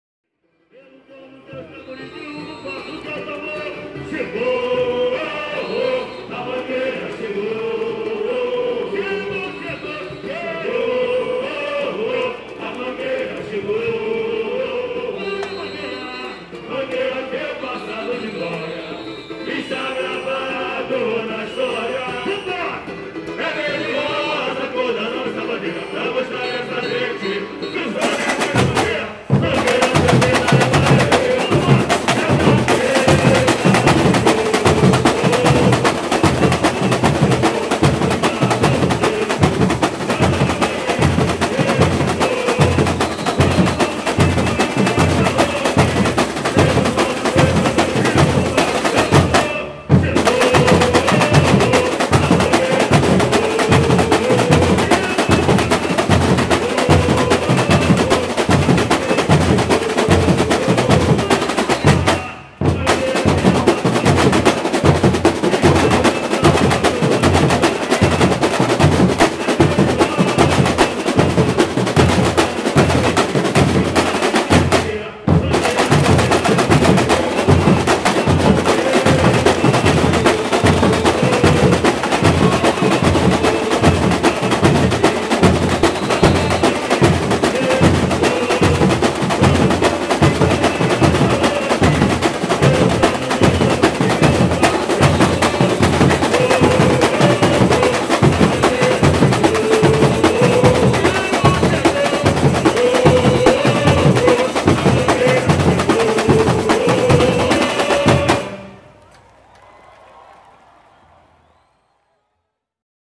Mangueira 2016 - Bateria (Esquenta) - Ensaio Técnico
Bateria da Mangueira em seu ensaio técnico em 20-12-15.